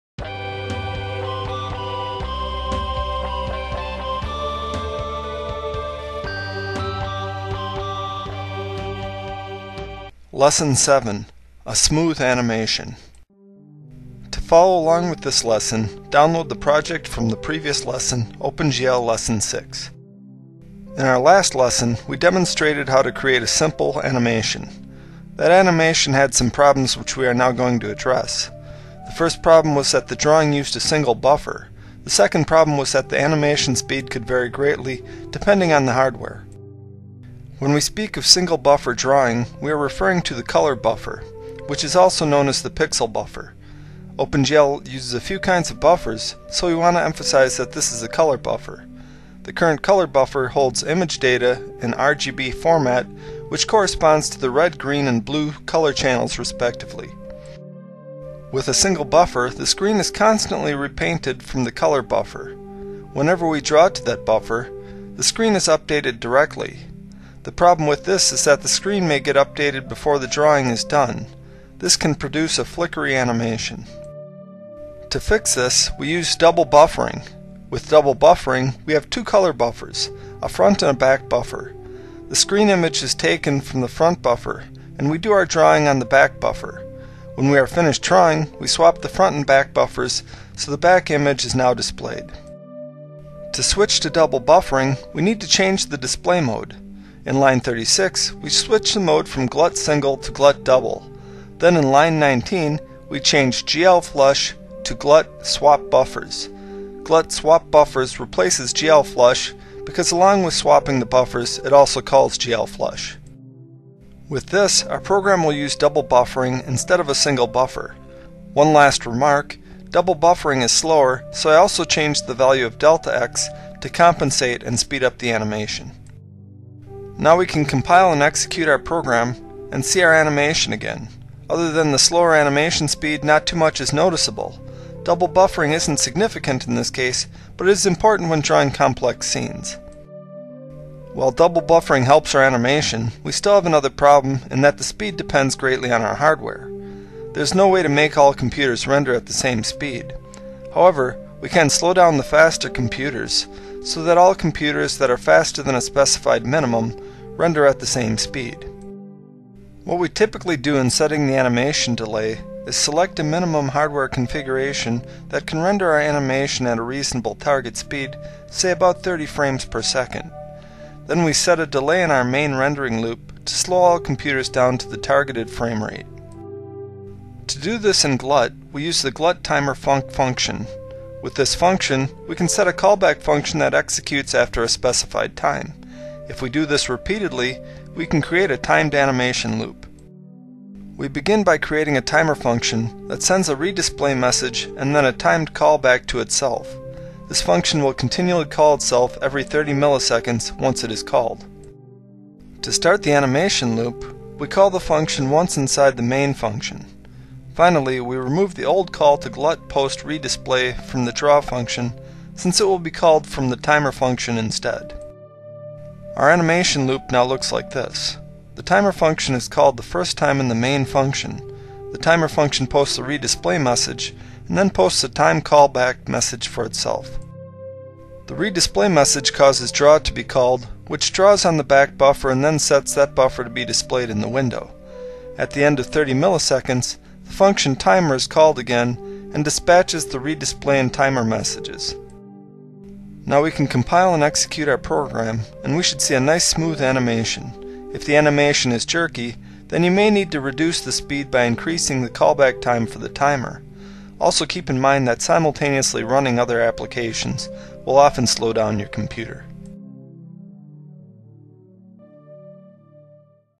C++ OpenGL Lesson 7: A Smooth Animation
For this C++ video lesson, we demonstrate how to create a smooth animation using OpenGL and GLUT and modifying the code from OpenGL Lesson 6. Our animation consists of a single line segment to which we have added double buffering and timing.